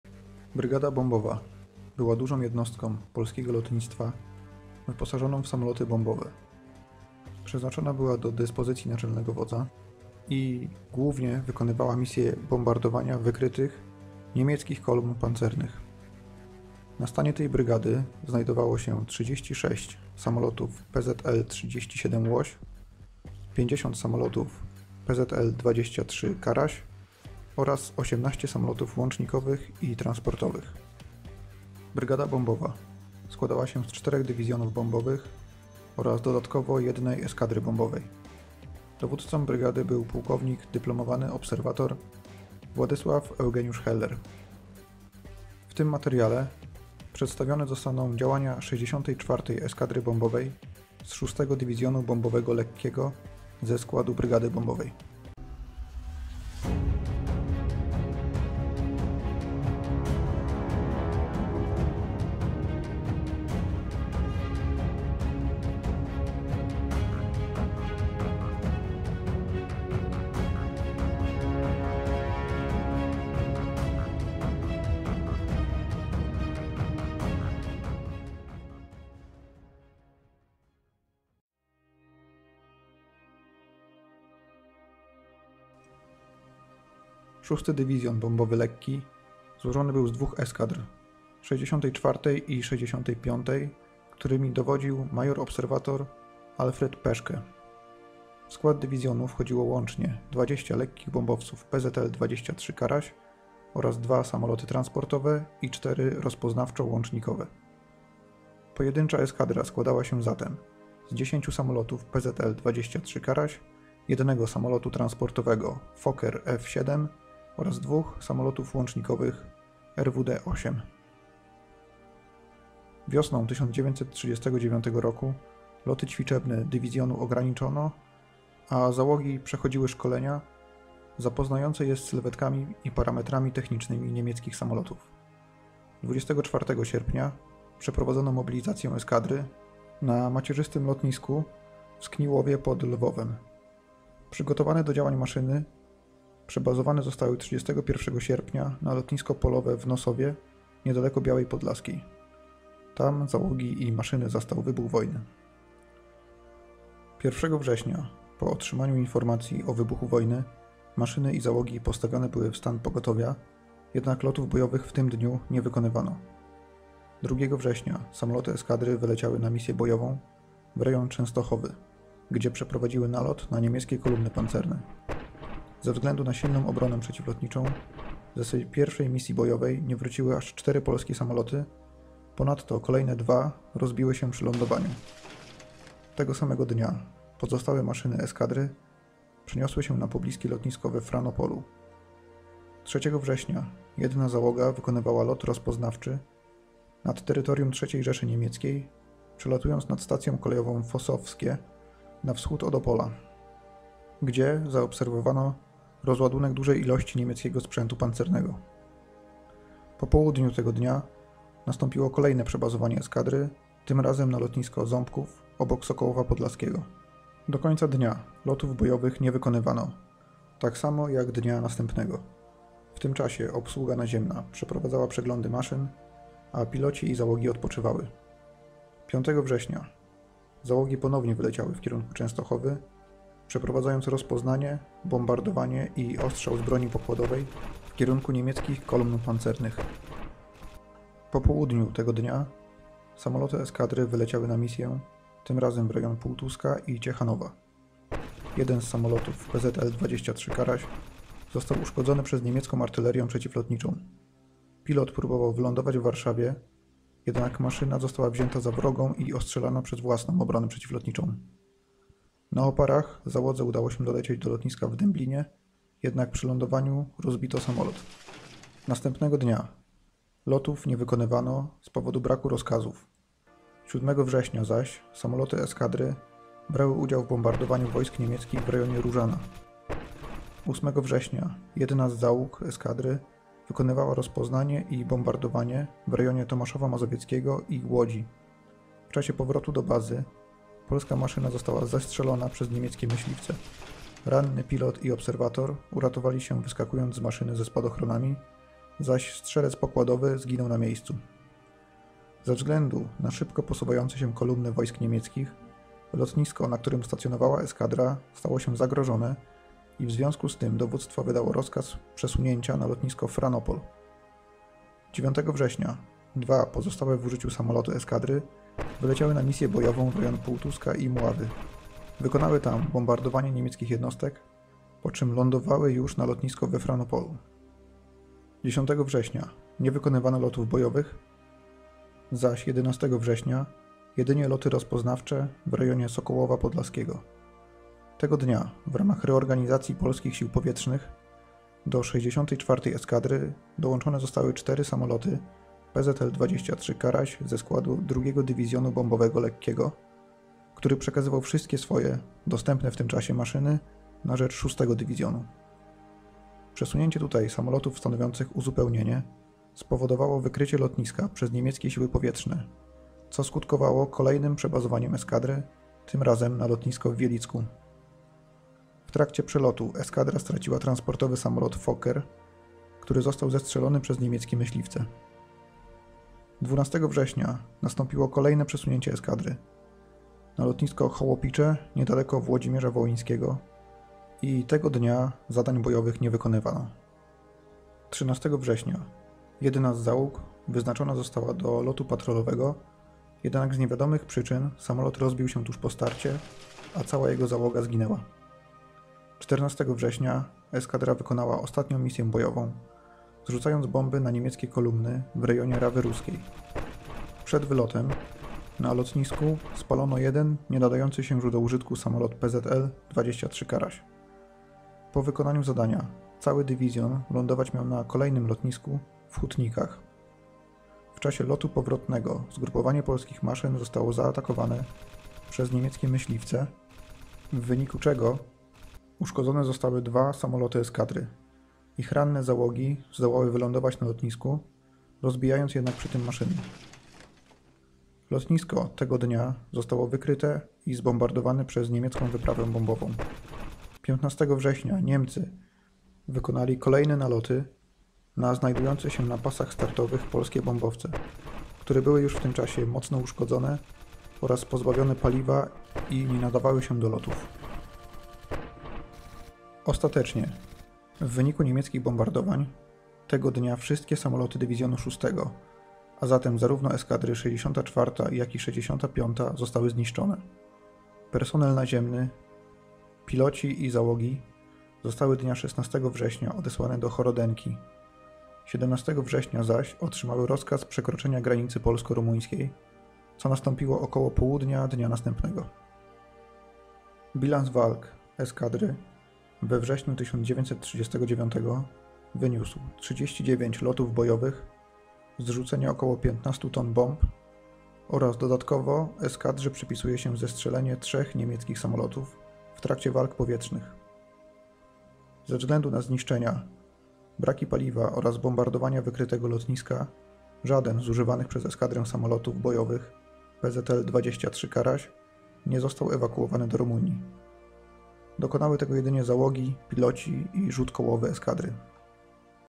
Polskie lotnictwo bombowe we wrześniu 1939. Działania 64 eskadry wyposażonej w samoloty PZL.23 Karaś
Eskadra nr 64 - szlak bojowy we wrześniu 1939.
Witajcie!
Dzisiejszy materiał jest "bliźniaczy" do jednego z już opublikowanych. Przedstawione są w nim działania polskiego lotnictwa bombowego w czasie wojny polsko - niemieckiej, tym razem na przykładzie szlaku bojowego 64 eskadry bombowej wyposażonej w samoloty PZL.23 Karaś.
Dzięki!